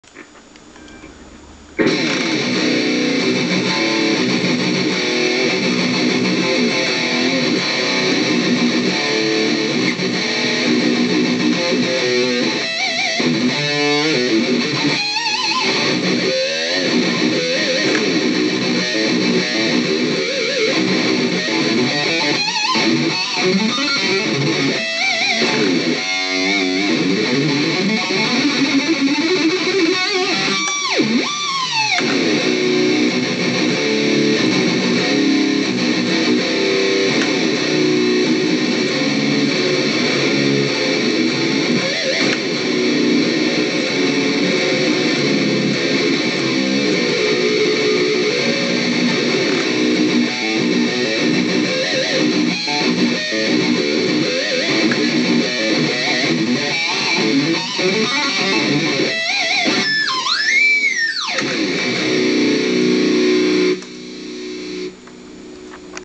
Cheesy metal riff
Jammin in a zakk wylde style,having a buzz,hope you like it!